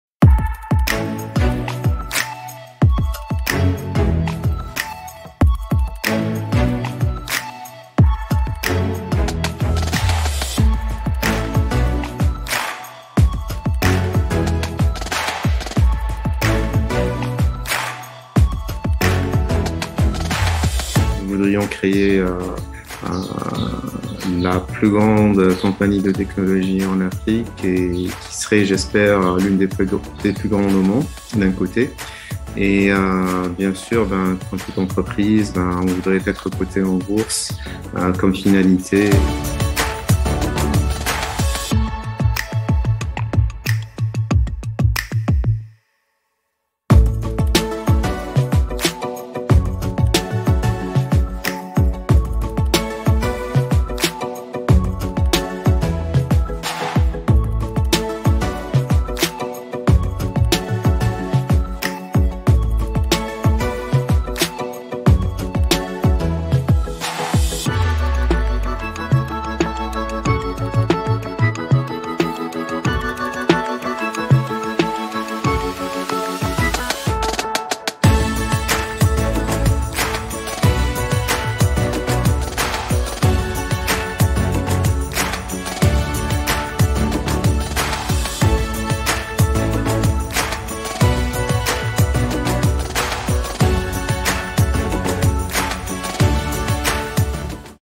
Le Uber Algérien, Yassir, à la conquête du monde 🇩🇿🚕
Le Uber 100% Algérien, Yassir, grandit au pas de course ! Entre service VTC, livraison de plats, la start-up se lance dans l’épicerie en ligne !  Vous avez déjà utilisé l'application Yassir ? 🇩🇿🚕